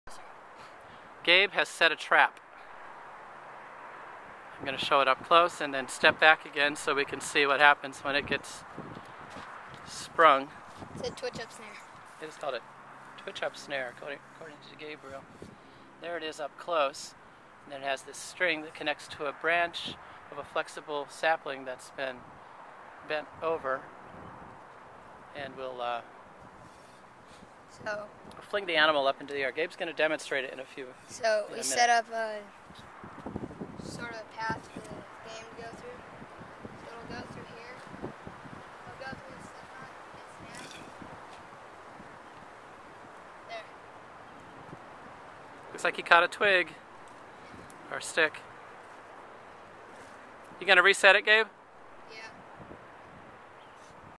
twitch-up snare trap
a twitch-up snare trap that Gabriel built in the wilderness